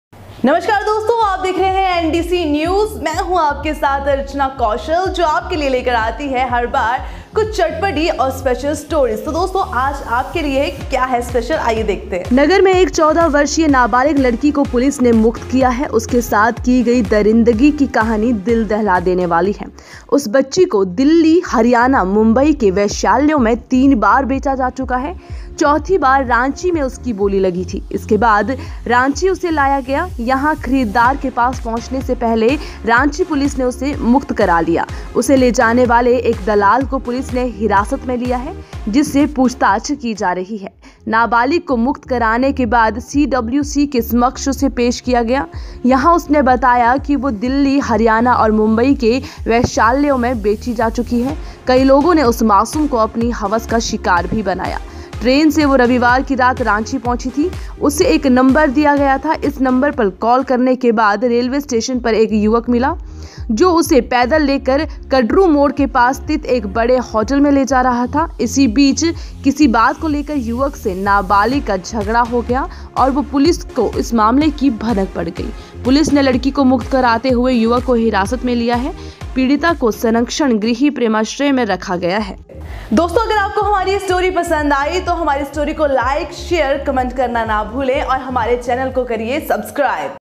14 साल की लड़की की दर्दभरी दास्तां, तीन वेश्यालयों के बाद चौथी बार रांची में बिकी नाबालिग
14 साल की लड़की की दर्दभरी दास्तां, तीन वेश्यालयों के बाद चौथी बार रांची में बिकी नाबालिग